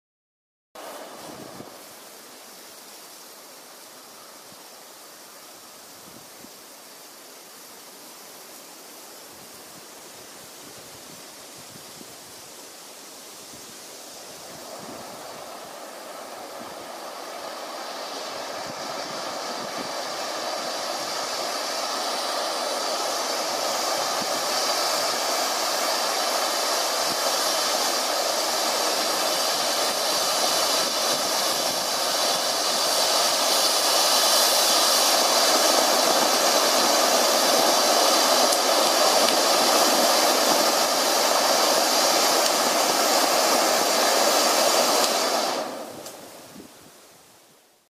Zink Vulkan No 5
Am größten, am höchsten, am schönsten, dass war unser Eindruck der Zink-Vulkane. Hier in der Ausführung "Gold mit Blauen Sternen".

Der Vulkan überzeugt durch Perfektion in Herstellung und Verarbeitung! Die Brenndauer des Vulkans beträgt ca. 50sek. Die Effekthöhe steigert sich bis auf 6m. Mit 250g Nettoexplosivmasse ist die zulässige Effektmasse komplatt ausgreizt - mehr geht nicht!